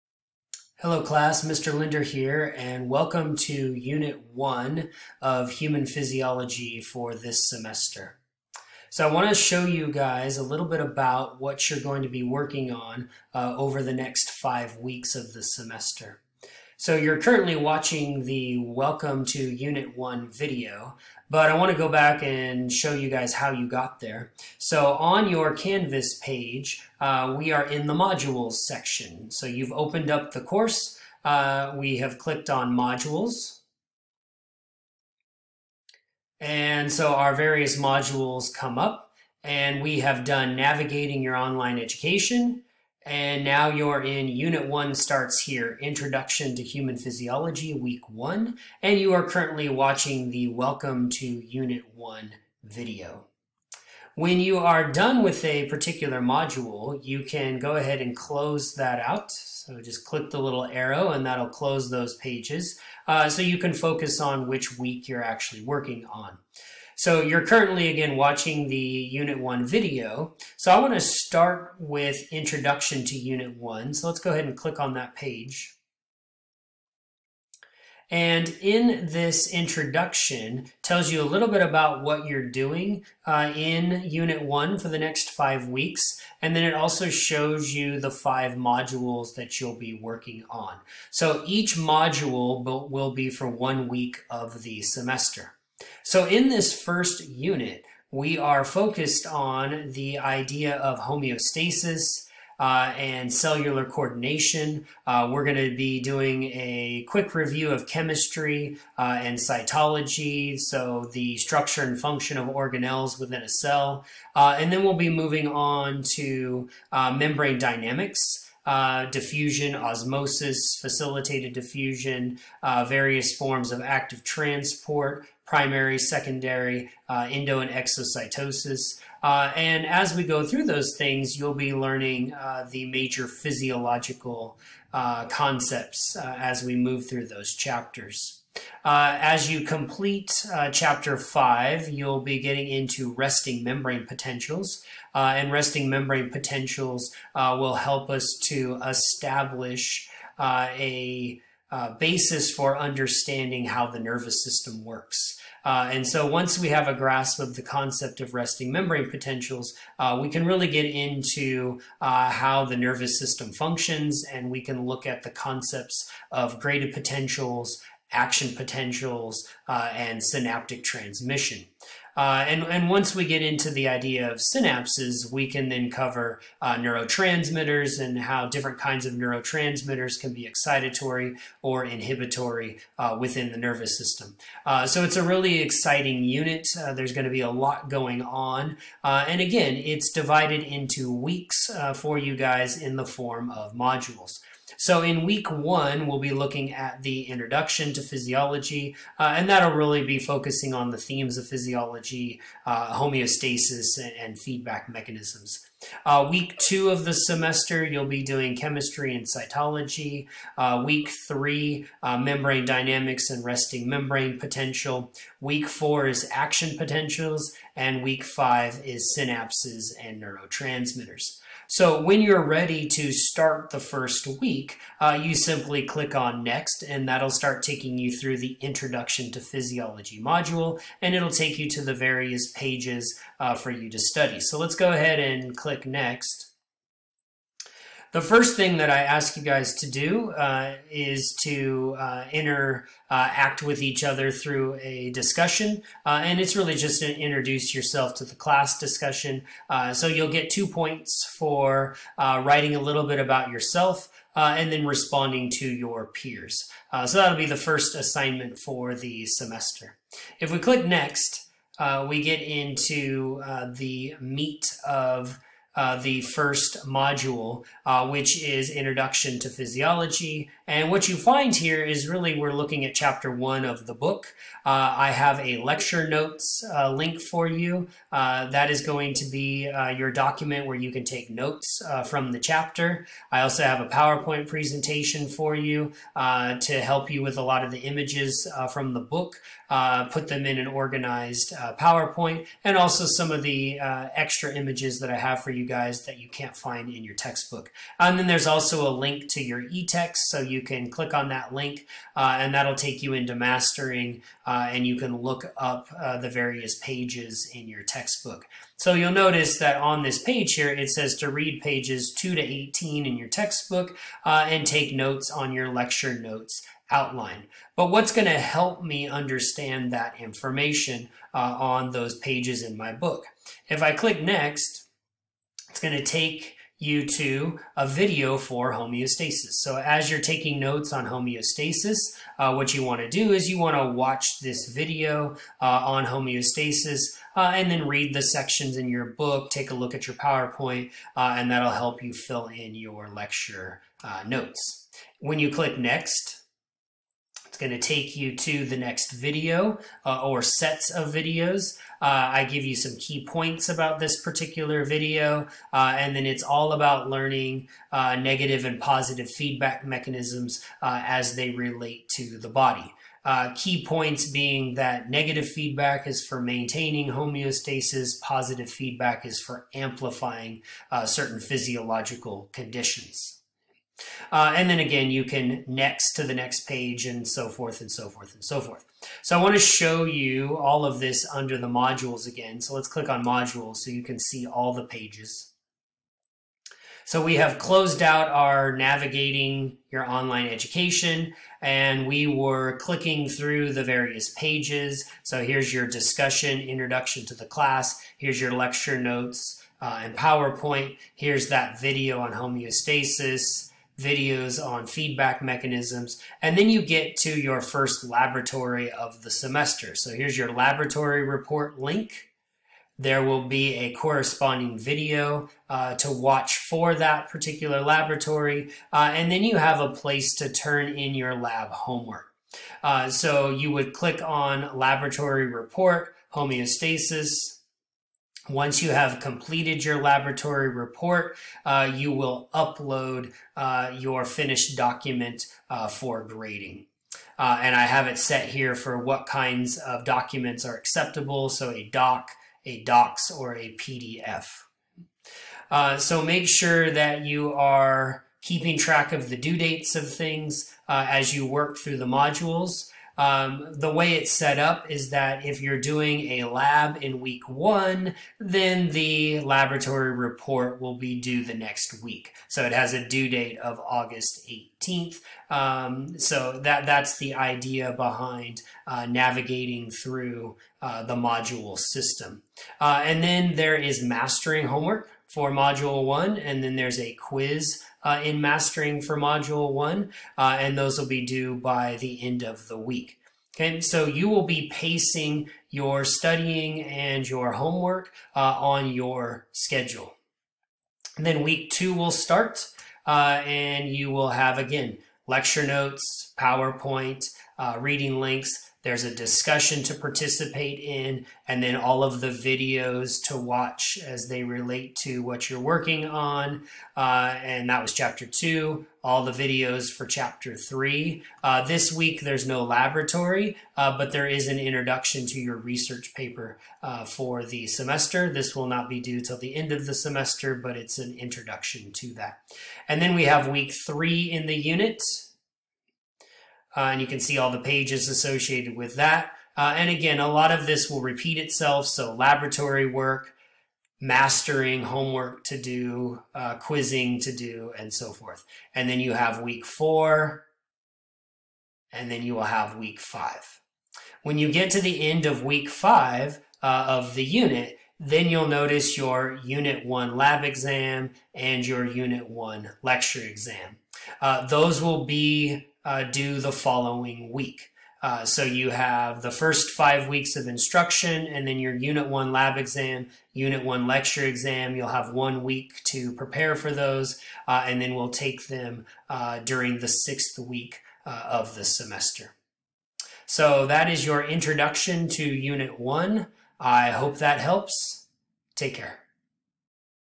Welcome to Unit 1 Biology 22
This video shows how to navigate the modules for Unit 1 of the semester.